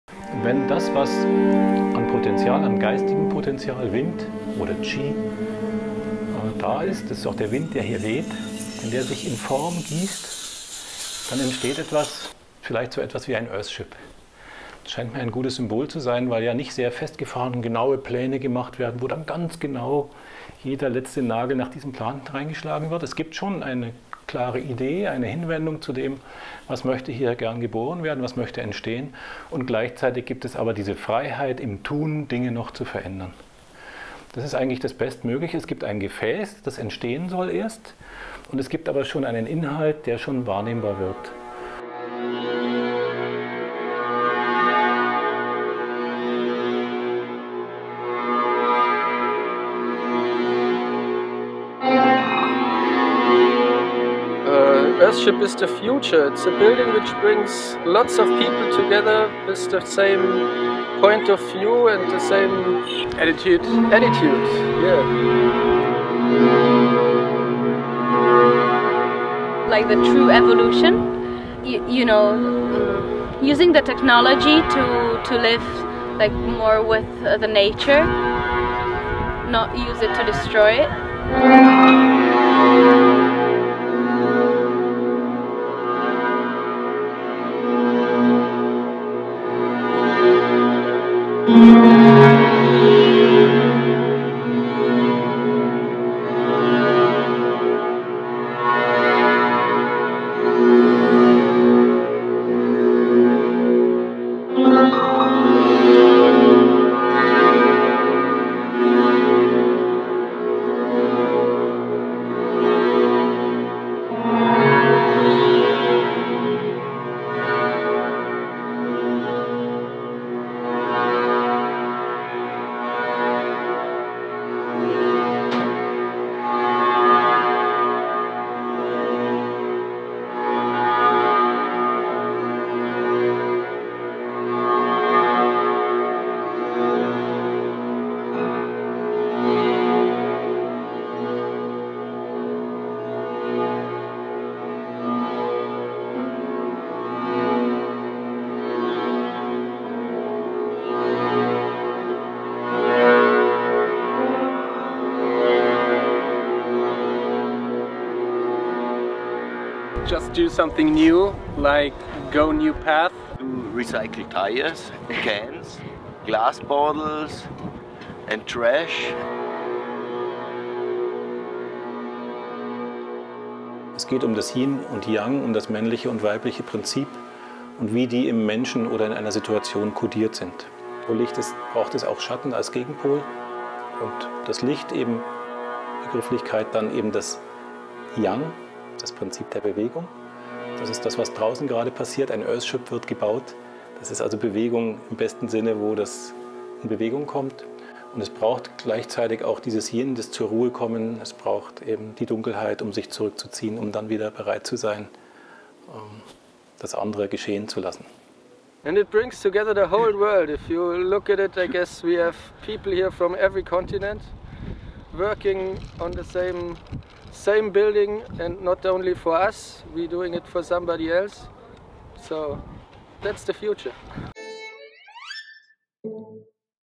FFCH // First Earthship in Germany Day 4
Today we met Jan Engbert and he made a song ffor the eartship...
Camera and edit (on the train): Sebastian Rost